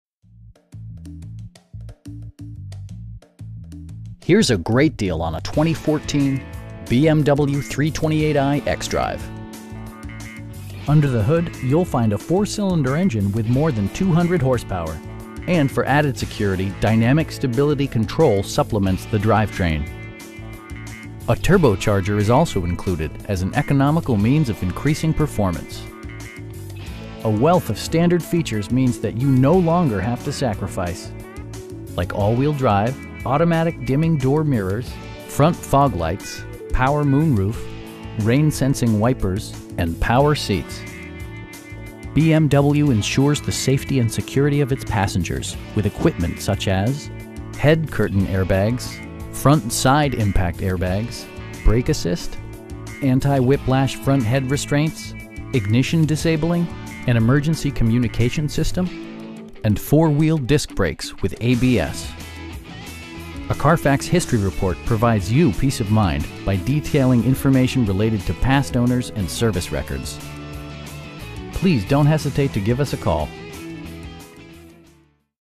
2014 BMW 3 Series 328i Xdrive Gran Turismo
Autobahn USA
88 Turnpike Rd

Here's a great deal on a 2014 BMW 328i xDrive. With just over 30,000 miles on the odometer, this model delivers an exhilarating ride without compromising ultimate luxury! Under the hood you'll find a 4 cylinder engine with more than 200 horsepower, and for added security, dynamic Stability Control supplements the drivetrain. A turbocharger is also included as an economical means of increasing performance. A wealth of standard features means that you no longer have to sacrifice. Like all wheel drive, 1-touch window functionality, a trip computer, automatic dimming door mirrors, front fog lights, power moon roof, rain sensing wipers, and power seats. BMW ensures the safety and security of its passengers with equipment such as: head curtain airbags, front SIDE impact airbags, traction control, brake assist, anti-whiplash front head restraints, ignition disabling, an emergency communication system, and 4 wheel disc brakes with ABS. A Carfax history report provides you peace of mind by detailing information related to past owners and service records. Our aim is to provide our customers with the best prices and service at all times. Please don't hesitate to give us a call.